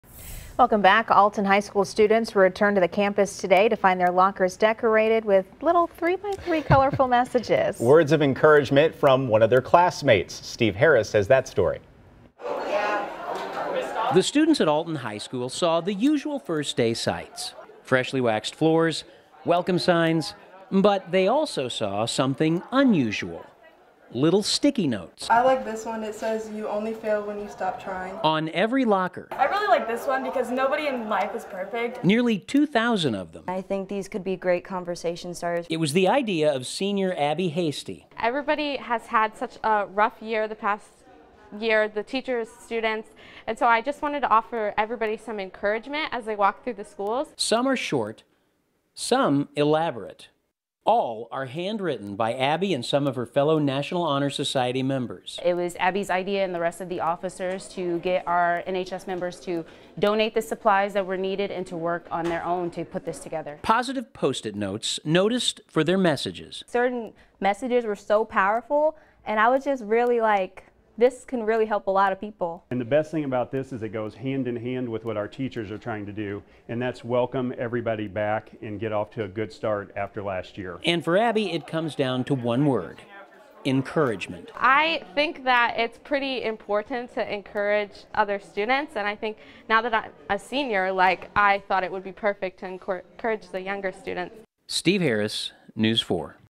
Alton HS senior coordinates nearly 2,000 notes of support for students as school resumes
The students at Alton High School saw the usual first-day sights of freshly waxed floors and welcome signs, but also saw something unusual: little sticky notes. They were placed on every locker, nearly 2000 of them, before school opened.